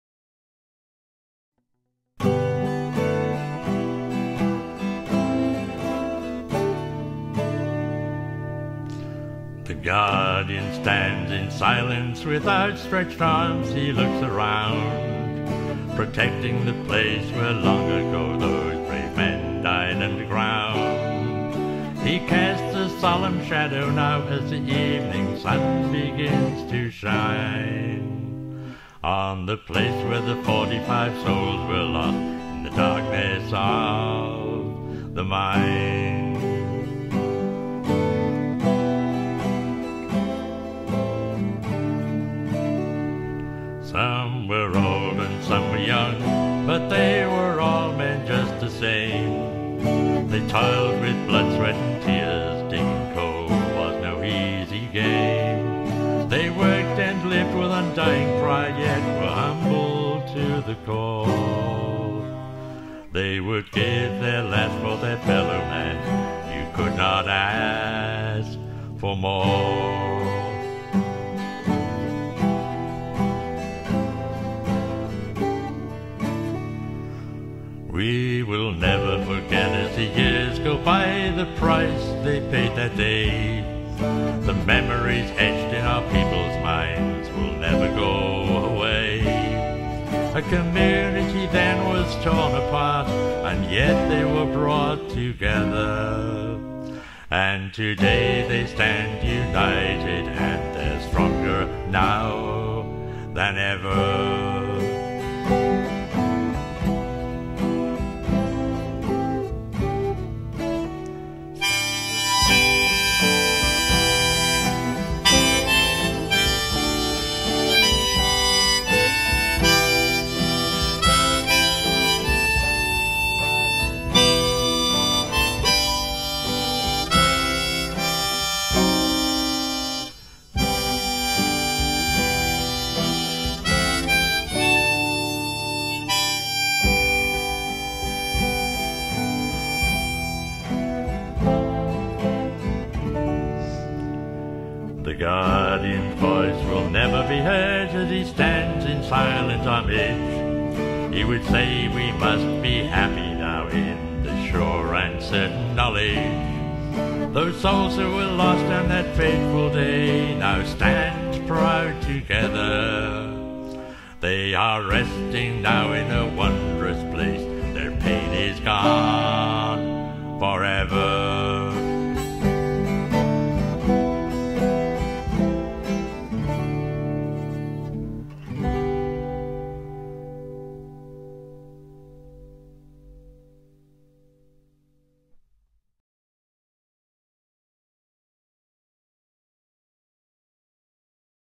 The Guardian
Poem by Ian Jones, ex-miner, written as tribute to The Guardian statue, erected in memory of the Six Bells Colliery Explosion June 28th 1960.

Music Written & Performed by Steve Webb

Recorded & Mastered by Jamyama Studios

Video content by Anytime Films Limited